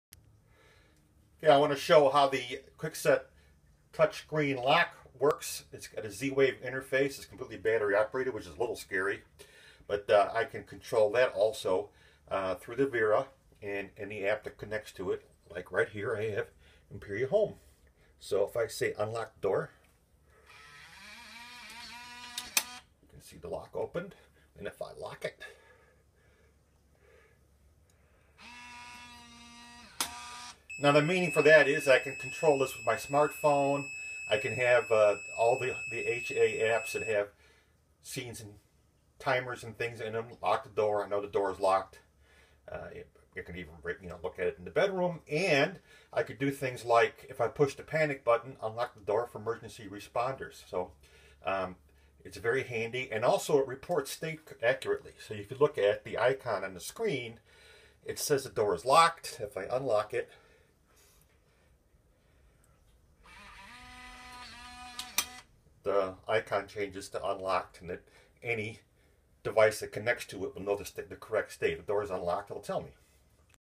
Kwikset 916 Smartcode lock control with ImperiHome on Android
From my Uniforum Chicago Home Automation presentation on February 27, 2017. A demonstration of the ImperiHome Application on Android controlling my Kwikset 916 Smartcode deadbolt door lock.